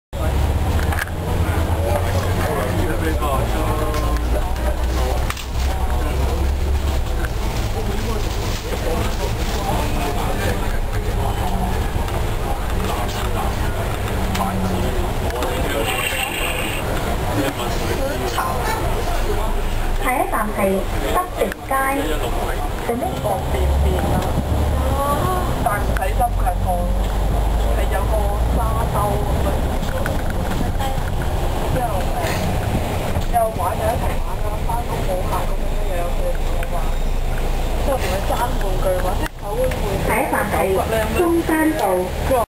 Nathan Road Hong Kong at night
Nathan Road from the front of a double decker bus